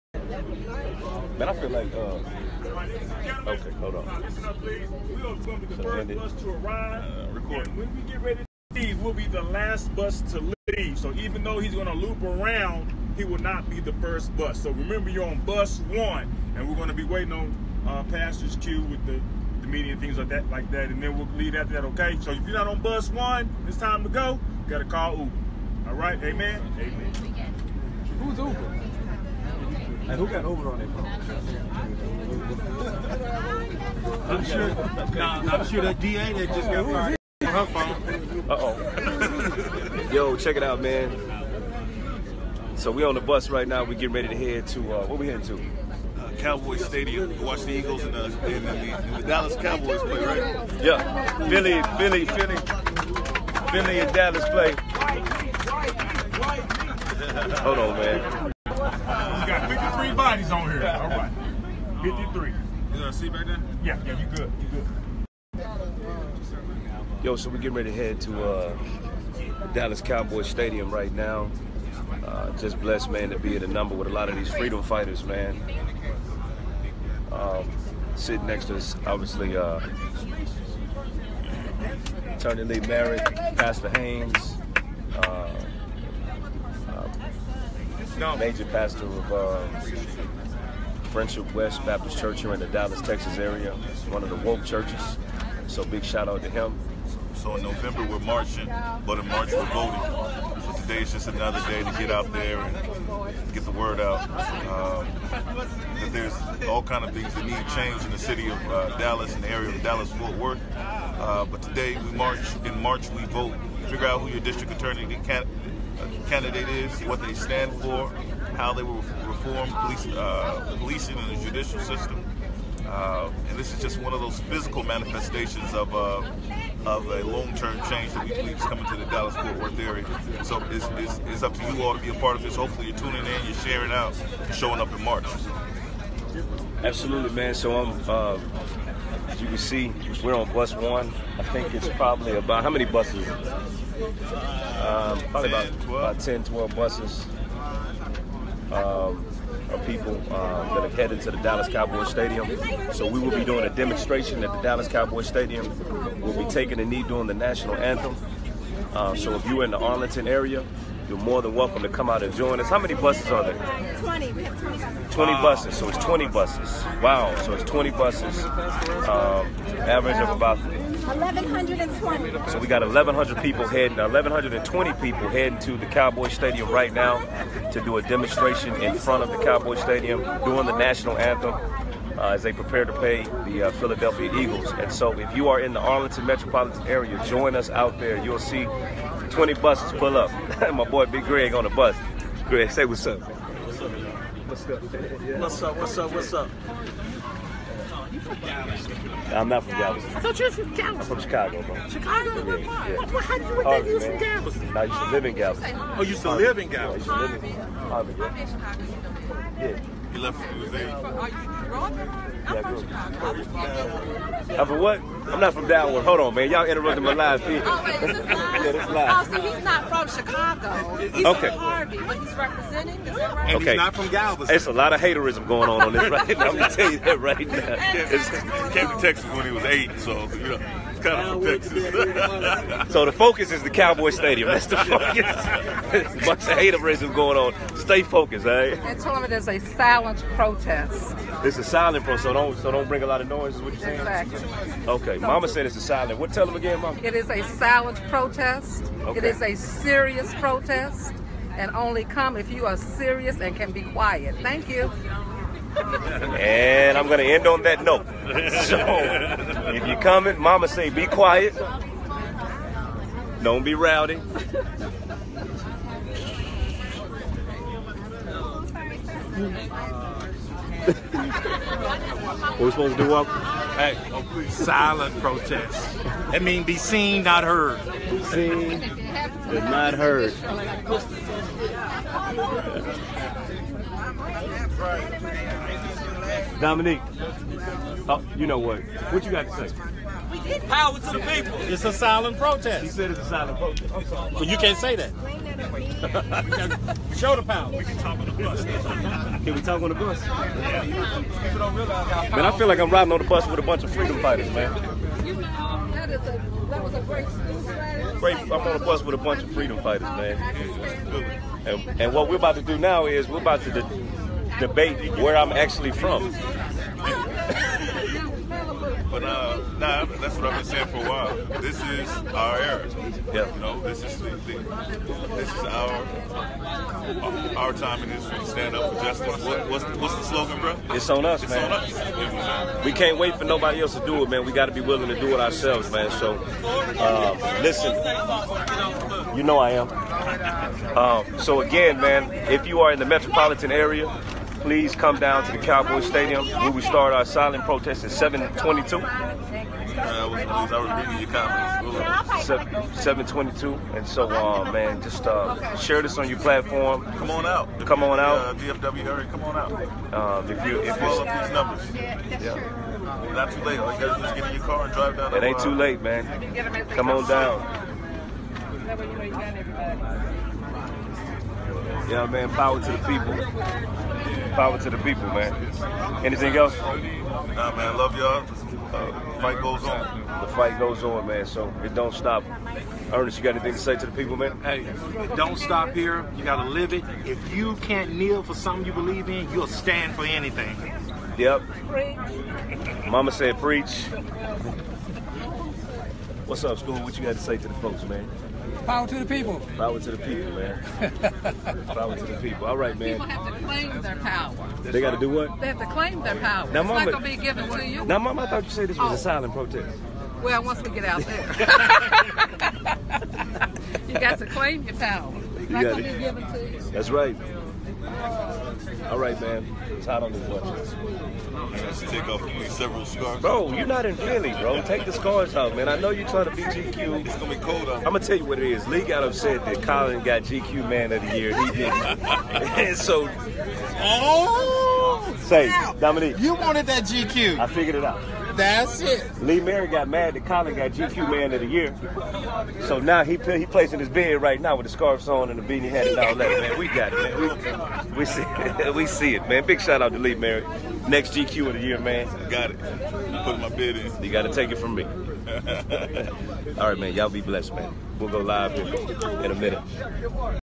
Lee Merritt & Cory Hughes headed to Dallas Cowboys stadium to take a knee
Also pastor freddie haynes from friendship baptist church is with them